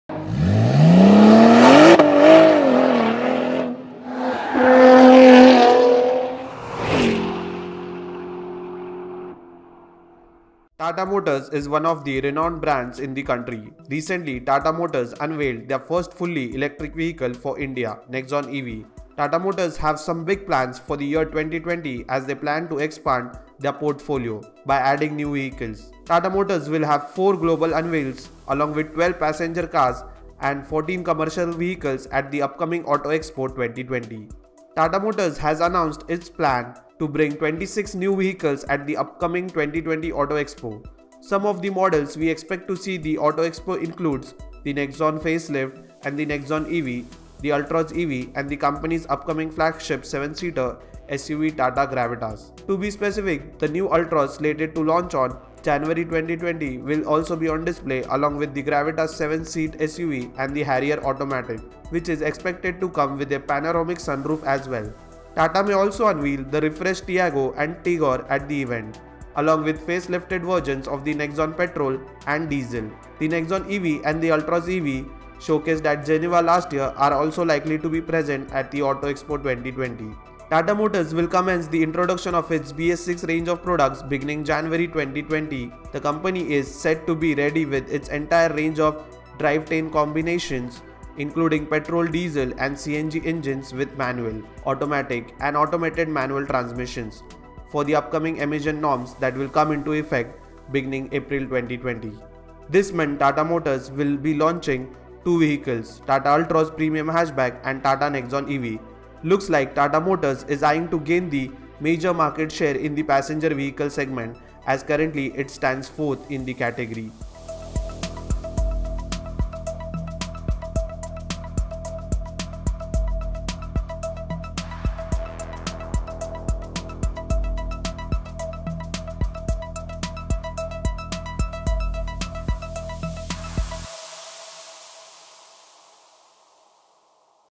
Tata Motors To Showcase 26 Models At The Upcoming Auto Expo 2020
Tata Motors is one of the renowned brands in the country, recently Tata Motors unveiled their first fully electric vehicle for India, Nexon EV. Tata Motors have some big plans for the year 2020 as they plan to expand their portfolio, by adding new vehicles.Tata Motors will have 4 global unveils, along with 12 passenger cars, and 14 commercial vehicles at the upcoming Auto Expo 2020.
Tata Motors has announced its plan to bring 26 new vehicles at the upcoming 2020 Auto Expo. Some of the models we expect to see at the Auto Expo include the Nexon facelift and the Nexon EV, the Altroz EV, and the company's upcoming flagship 7-seater SUV Tata Gravitas.

To be specific, the new Altroz, slated to launch on January 22, will also be on display, along with the Gravitas seven-seat SUV and the Harrier automatic, which is expected to come with a panoramic sunroof as well. Tata may also unveil the refreshed Tiago and Tigor at the event, along with facelifted versions of the Nexon petrol and diesel. The Nexon EV and the Altroz EV (showcased at Geneva last year) are also likely to be present at the Auto Expo 2020.

Tata Motors will commence the introduction of its BS6 range of products beginning January 2020. The company is said to be ready with its entire range of drivetrain combinations, including petrol, diesel and CNG engines with manual, automatic and automated-manual transmissions for the upcoming emission norms that will come into effect beginning April 2020.

This month Tata Motors will be launching two vehicles Tata Altroz premium hatchback and Tata Nexon EV, Looks like Tata Motors is eyeing to gain the major market share in the passenger vehicle segment, as currently it stands 4th in the category.